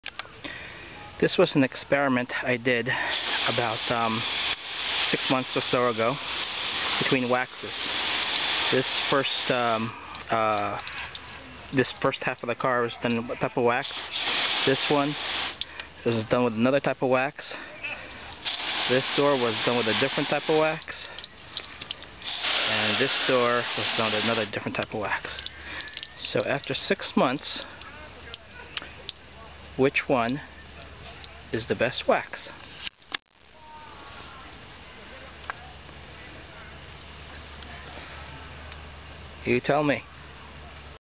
Klasse High Gloss Sealant Glaze vs. Zaino Z-5 PRO Show Car Polish
This is a comparison of how long each was (or sealant/polish in this case) compares in about 6 months of use.  The hood/front quarter panel and the 2nd set of doors were done with the Zaino Z-5 PRO.  The 1st set of doors and the rear quarter panel was done with the Klasse.  Which would you rather want on your car?  I was a huge fan of Klasse and used it for several years.  I now found a new product admittedly expensive at $17 per 8 oz vs. the Klasse at $20 per 16.9 oz.  Or $2.13/oz Zaino vs. $1.18/oz for the Klasse.  I'm not even counting the shipping cost for the Zaino...